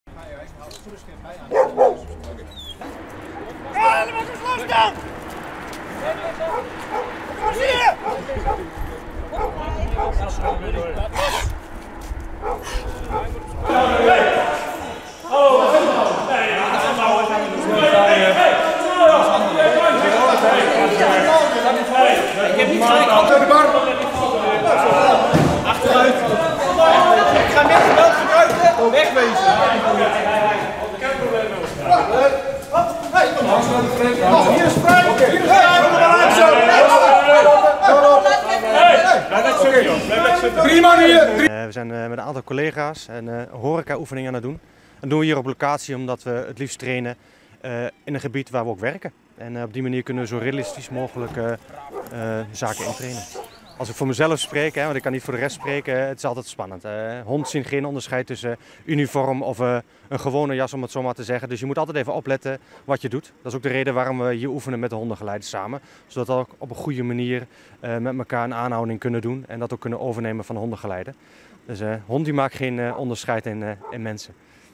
VIDEO: Opnieuw politie bij Club Déjàvu, maar nu voor een oefening
DINXPERLO - Het was er weinig avonden rustig: Club Dejavu in Dinxperlo. Er waren vrijwel altijd ongeregeldheden en de discotheek werd uiteindelijk door burgemeester Anton Stapelkamp gesloten.